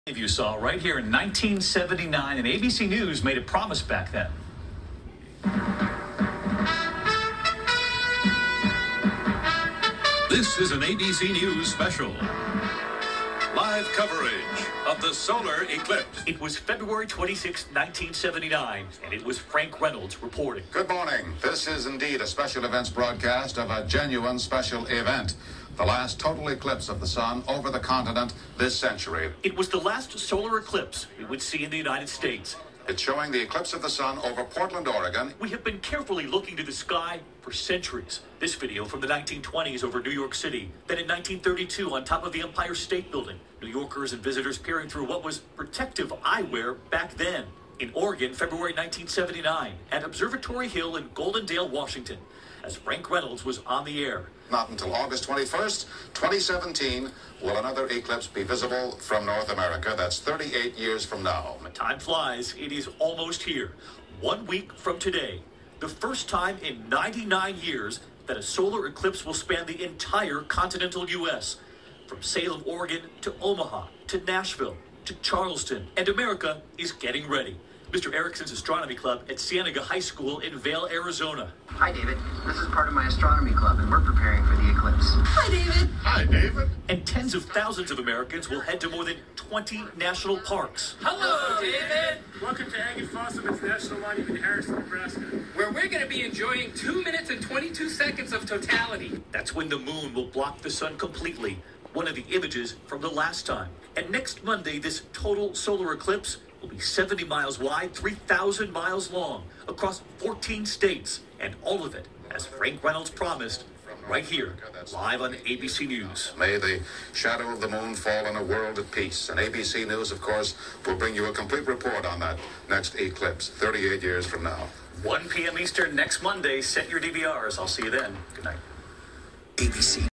ABC news look back at Solar Eclipse coverage from 1979
ABC news look back at Solar Eclipse coverage from 1979 and a look forward at the solar eclipse in August 2017